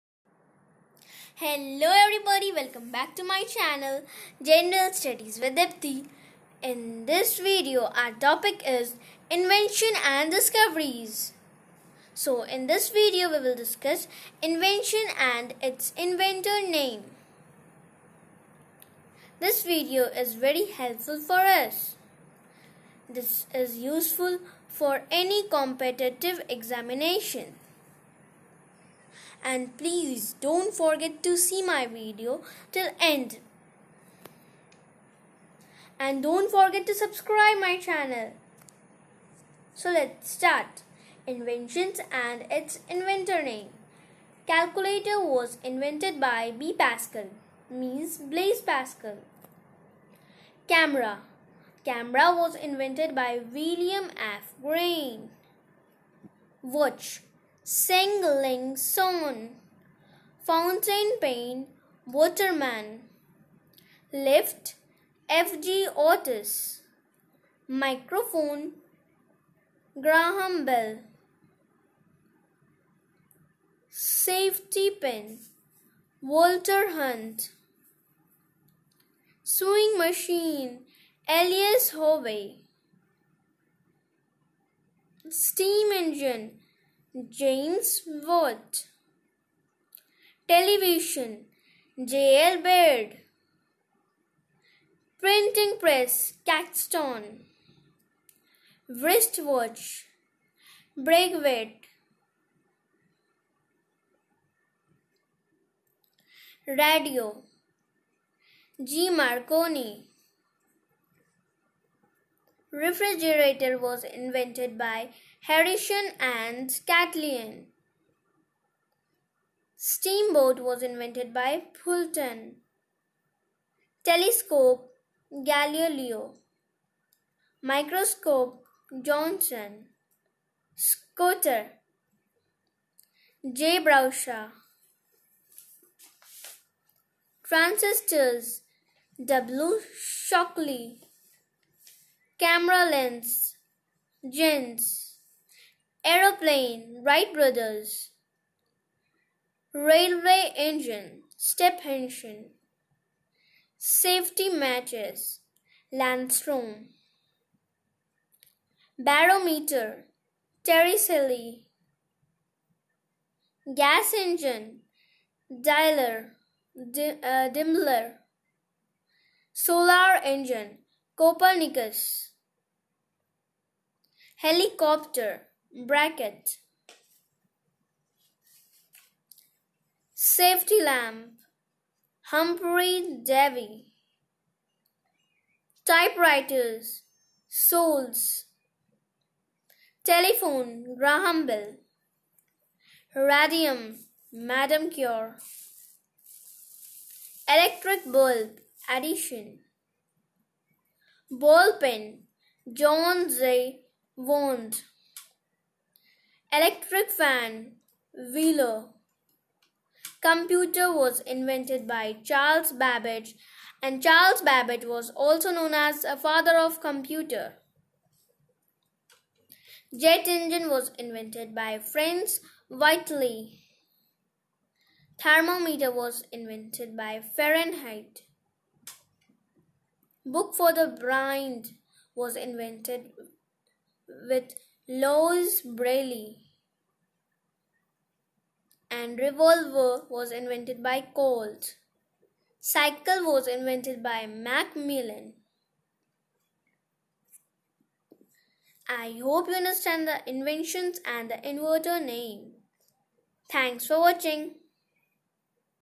top 40 inventions and inventors || For competitive exams || by General studies with deepti
In this video I had discussed about the top 40 inventions and inventors.
Hope u all enjoyed the video 😊😊😊😊😊